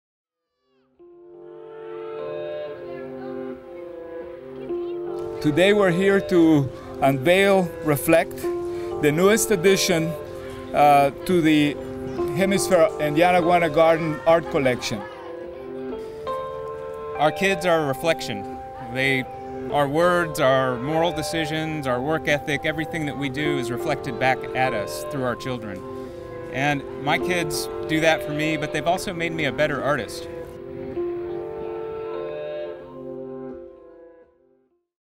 “Reflect” Public Art Dedication At Yanaguana Garden - March 2, 2018 (30-sec. version)
"Reflect" was commissioned by the City of San Antonio, and is the work of local artist Stuart Allen. Reflect is the eighth and final public art installation of the series “Play” at Yanaguana Garden created by local artists, and led by Allen. “Play” represents a collaboration between the City of San Antonio and the Hemisfair Park Area Redevelopment Corporation (HPARC) in a continuation of their commitment to bring beautiful public art to the redeveloping downtown district.

“Reflect” is sensitively integrated into the playground area of Yanaguana Garden, creating a canopy over the existing “boomerang” playground element. The sculpture is a series of stainless steel ribbon forms suspended from a robust arched frame fabricated from bent steel tubing. Its organic shape harmonizes with the adjacent promenade’s shade structure design.